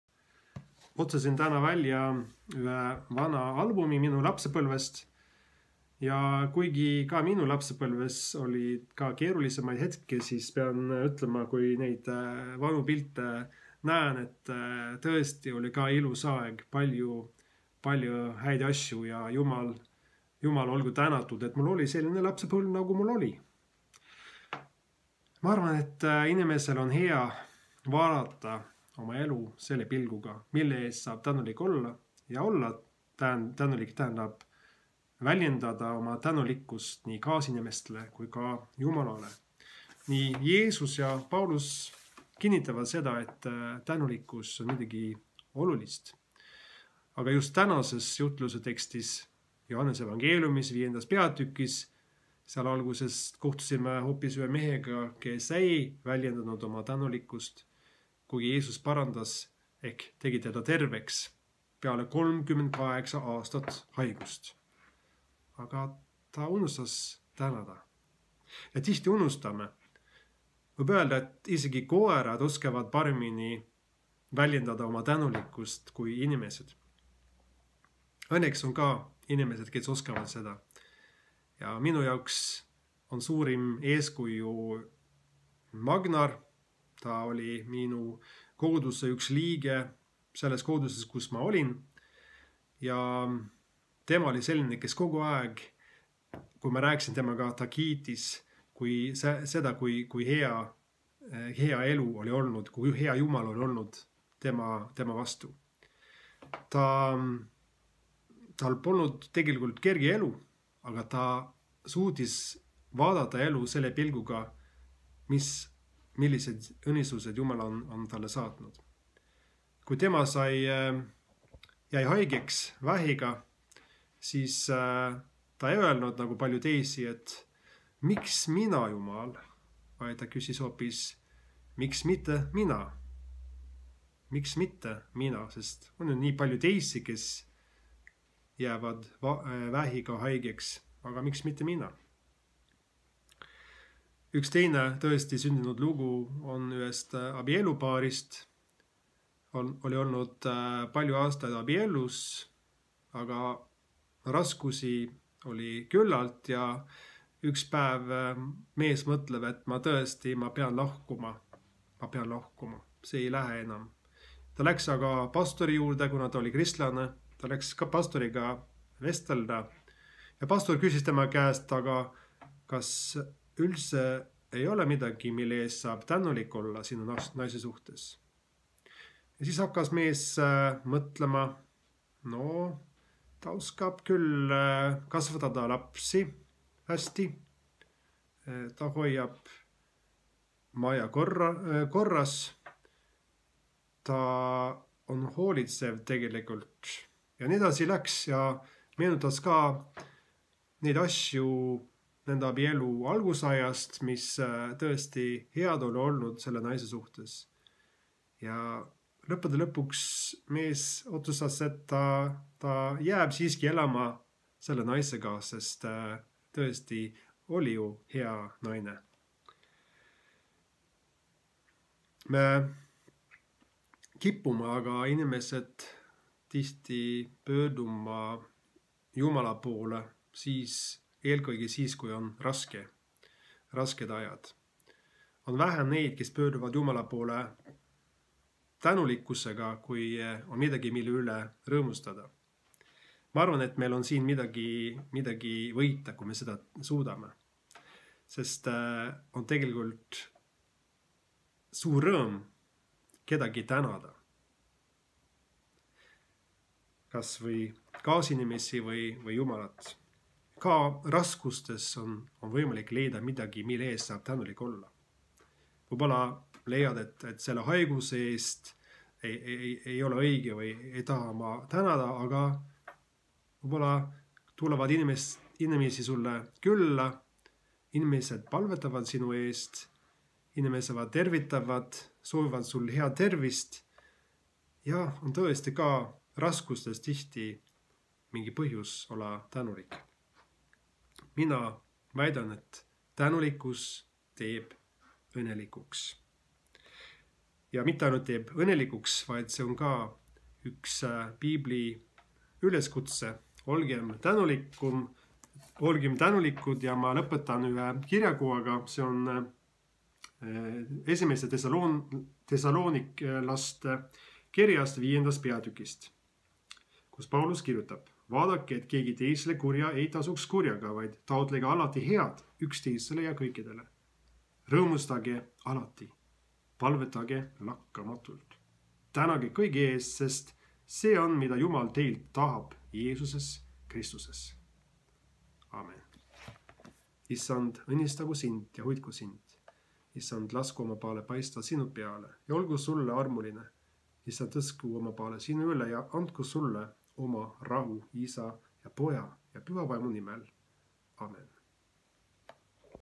«Tänulikkus teeb ōnnelikuks». Minijutlus 15. pühapäeval pärast nelipüha, 13.09.2020.
EELK Saku Toomase koguduse pastori jutlus 13.09